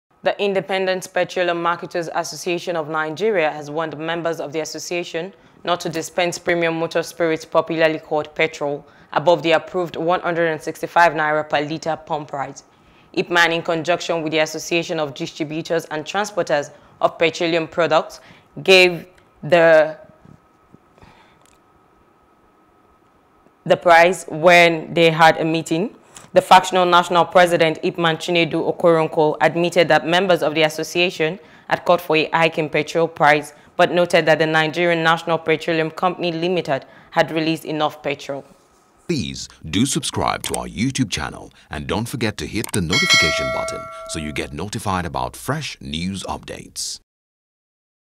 Fuel Crisis: Don’t Sell Fuel Above N165/Litre, I.P.M.A.N. Warns Members | NEWS
The Independent Petroleum Marketers Association of Nigeria has warned members of the association not to dispense Premium Motor Spirit, popularly called petrol, above the approved N165/litre pump price.